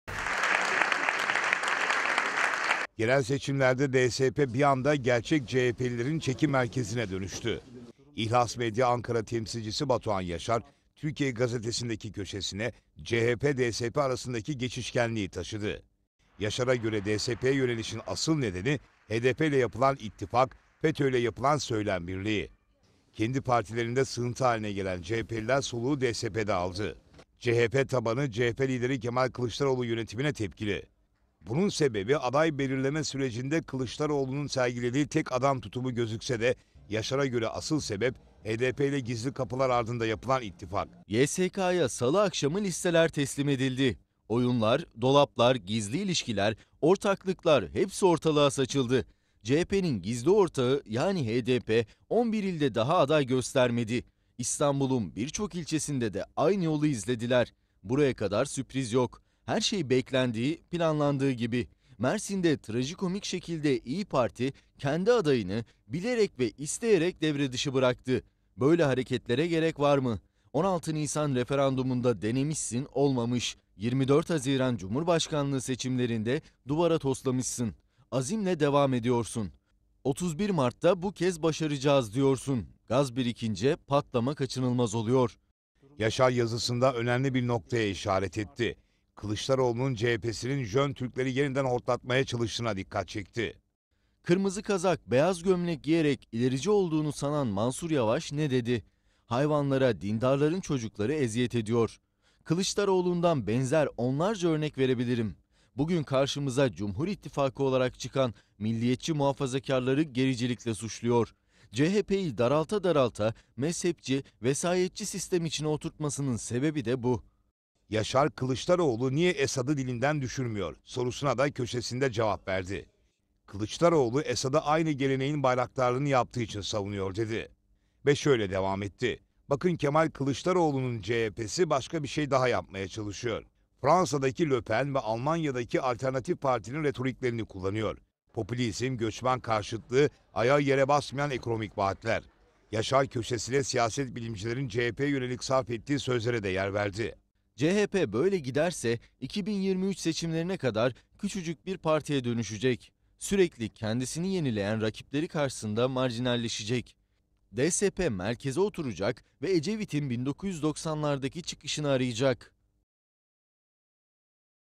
Jön Türkler, HDP ve CHP olayı mı? 21.02.2019
Jön Türkler, HDP ve CHP olayı mı?

Tarih tekerrürden ibaret..
Yakın ve uzak tarih için de aynı şey pekala söylenebilir..
YSK’ya Salı akşamı listeler teslim edildi..
Oyunlar, dolaplar, gizli ilişkiler, ortaklıklar hepsi ortalığa saçıldı..
CHP’nin “gizli ortağı” yani HDP 11 ilde daha aday göstermedi..
İstanbul’un bir çok ilçesinde de aynı yolu izlediler..
Buraya kadar sürpriz yok..
Her şey beklendiği, planlandığı gibi..
Mersin’de trajikomik şekilde İYİ parti kendi adayını “bilerek ve isteyerek” devre dışı bıraktı..
Böyle hareketlere gerek var mı?
16 Nisan Referandumunda denemişsin olmamış..
24 Haziran Cumhurbaşkanlığı seçimlerinde duvara toslamışsın..
Azimle devam ediyorsun..
“31 Mart’ta bu kez başaracağız” diyorsun..
Gaz birikince patlama kaçınılmaz oluyor..
CHP-DSP arasındaki geçişkenliği iyi izlemek gerekir..

DSP VE “ORTANIN SOLU”
Ecevit, İnönü’ye bayrak açmıştı..
O yıllarda kullandığı ifade sanki bugünlerin CHP’si için söylenmişti:
“Ortanın Solu”..
Ecevit neden “ortanın solu” dedi?
-CHP halka yabancılaştığı,
-Dışarıdan yönlendirildiği,
-İçindeki bürokrasinin partiyi yemeye başladığı için..
Ecevit, İnönü’yü devirdi devirmesine ama..
CHP’nin “Jön Türkleri” zaman içinde Ecevit’i kapının önüne koymayı bildi..
CHP’den kaçarcasına gidip DSP’yi bu yüzden kurdu..
“Sol”u halkla barıştırdı..
Milleti kucakladı..
Sonra da iktidara geldi..
Gerisini anlatmaya gerek yok zaten..

KILIÇDAROĞLU NİYE ESAD’I DİLİNDEN DÜŞÜRMÜYOR?
İttihat-Terakki veya Jön Türkler..
Ta Abdülhamit Han’dan bu yana kirli oyunlarla Türkiye’ye tuzak kuruyor..
İçerden dışarıya bayrak sallayanları kastediyoruz..
Milleti hakir görüyorlar..
Statüko, bürokrasi ve askeri vesayetin yılmaz savunuculuğunu yapıyorlar..
İşte tam da bu yüzden iktidara gelemiyorlar..
Ortadoğu’daki Baas tarzı yapılanmaların bunlardan farkı yok..
Kendi halklarına zulmederler..
Toplumlarını gözlerini bile kırpmadan iç savaşa sürüklerler..
Kılıçdaroğlu, Esad’ı aynı geleneğin bayraktarlığını yaptığı için savunuyor..

“BİZ İLERİCİ, SİZ GERİCİ”
Kırmızı kazak, beyaz gömlek giyerek ilerici olduğunu sanan Mansur Yavaş ne dedi?
-“Hayvanlara dindarların çocukları eziyet ediyor..”
Kılıçdaroğlu’dan benzer onlarca örnek verebilirim..
Bugün karşımıza “Cumhur İttifakı” olarak çıkan milliyetçi-muhafazakarları gericilikle suçluyor..
Kılıçdaroğlu’nun CHP’si Jön Türkleri yeniden hortlatmaya çalışıyor..
CHP’yi daralta daralta ‘mezhepçi-vesayetçi sistem’ içine oturtmasının sebebi de bu..

CHP-HDP GEÇİŞKENLİĞİ
Son iki seçimde başka bir şeyle de yüzleştik..
HDP’den CHP’ye veya CHP’den HDP’ye yöneliş..
Bununla da kalmadı..
Söylem ve eylem birliği..
Kimse pek farkında değil ama TBMM’de CHP-HDP tek grup gibi hareket ediyor..
İşte “CHP seçmeni neden küstü?” sorusunun cevabı..
DSP’ye yönelişin asıl nedeni;
-HDP ile yapılan ittifak..
-FETÖ ile yapılan söylem birliği..
DSP, bir anda “gerçek CHP’liler”in çekim merkezine dönüştü..
Kendi partilerinde “sığıntı” gibi kaldıkları için..

CHP HIZLA MARJİNALLEŞİYOR
Siyaset bilimcilerden ilginç sözler duyuyoruz..
-“CHP böyle giderse 2023 seçimlerine kadar küçücük bir partiye dönüşecek”..
-“Sürekli kendisini yenileyen rakipleri karşısında marjinalleşecek”..
-“DSP merkeze oturacak ve Ecevit’in 1990’lardaki çıkışını arayacak”..

ERDOĞAN’IN SUÇU
Bakın Kemal Kılıçdaroğlu’nun CHP’si başka bir şey daha yapmaya çalışıyor..
Fransa’daki Le Pen ve Almanya’daki Alternatif Parti’nin retoriklerini kullanıyor..
-Popülizm..
-Göçmen karşıtlığı (Suriyeliler)
-Ayağı yere basmayan ekonomik vaatler..
Cumhurbaşkanı Erdoğan’ı “her şeyi yaptın ama bir şu muhalefeti dönüştüremedin” diye suçlayanlar var..
Aslında bu ne onun işi ne de konusu..

CHP İÇ BARIŞI TEHDİT EDİYOR
Bu talep CHP’nin sürekli sokağı dürtmesinden kaynaklanıyor..
Halk huzursuz..
Sokak çağrılarından rahatsız oluyor..
İşte DSP’ye yönelimin bir başka nedeni de bu..
Acaba DSP, İngiltere’deki işçi partisi, Almanya’daki sosyal demokratlar gibi olur mu?
Neden olmasın..
DSP’nin Türkiye’deki muhalefet eksikliğini doldurup dolduramayacağını birlikte göreceğiz..
31 Mart HDP ortaklığı CHP’nin şapkadan çıkardığı son tavşan olabilir..
CHP tabanı ve örgütü bile artık Kılıçdaroğlu’na tahammül edemiyor..
Türkiye 2023’e kadar huzur istiyor..